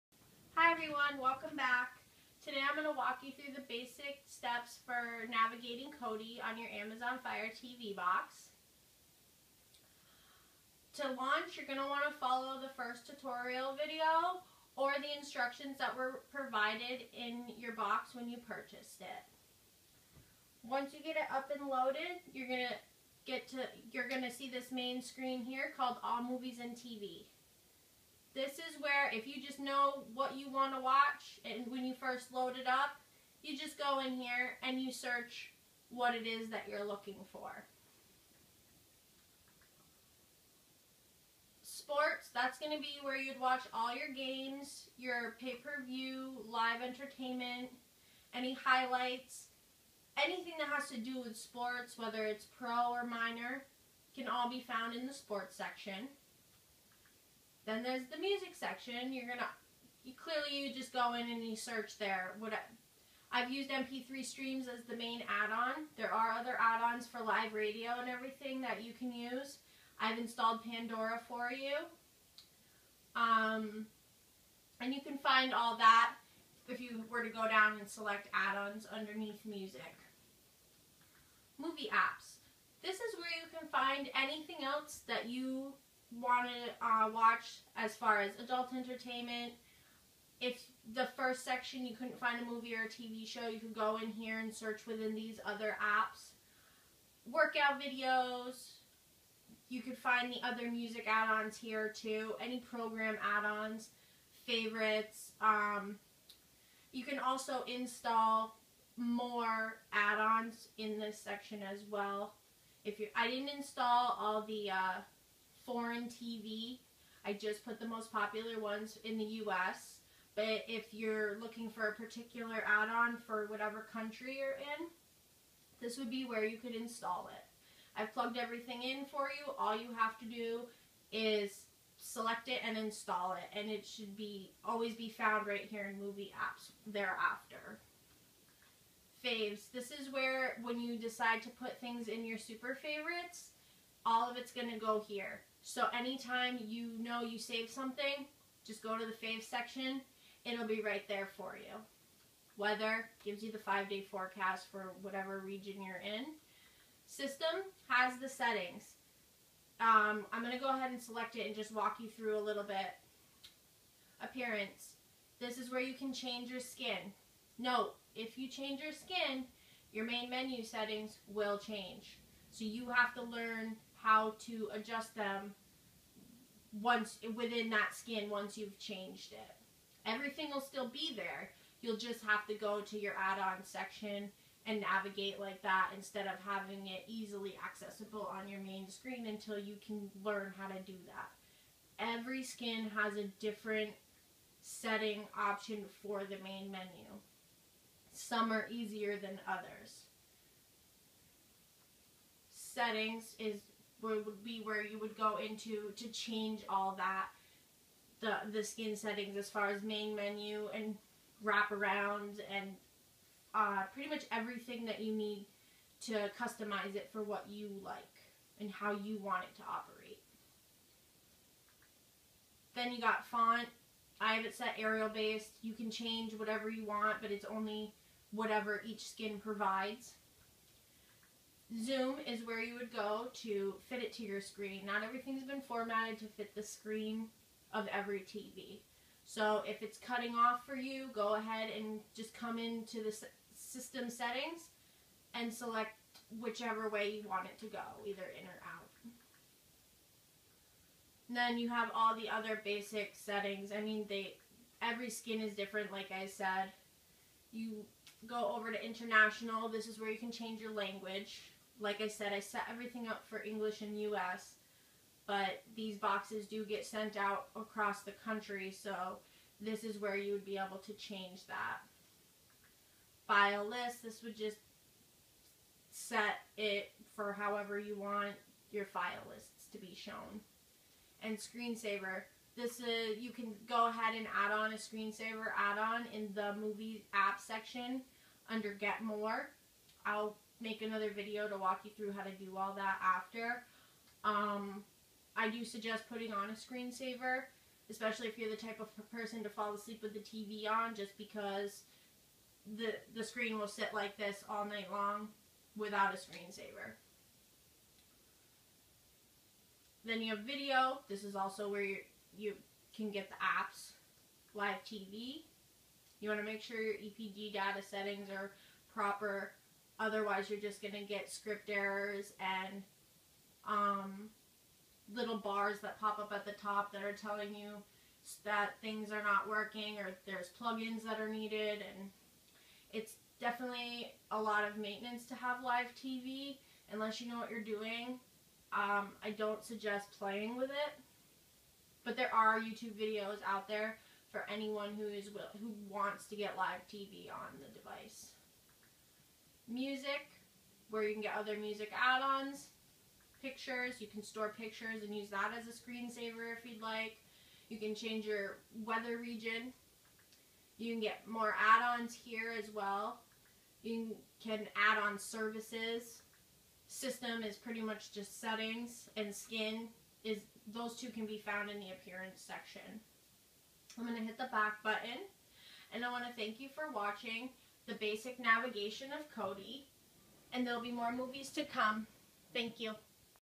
Basic Navigation of KODI on Amazon Fire TV
This video is for basic navigation and operations of KODI on Amazon Fire TV. The tutorial will help anyone who has purchased their Unlocked and Loaded Fire TV from our website. If you would like to Purchase an Unlocked and Loaded Fire TV Box or have questions; Please visit: http;//firetv-unlockedandloaded.com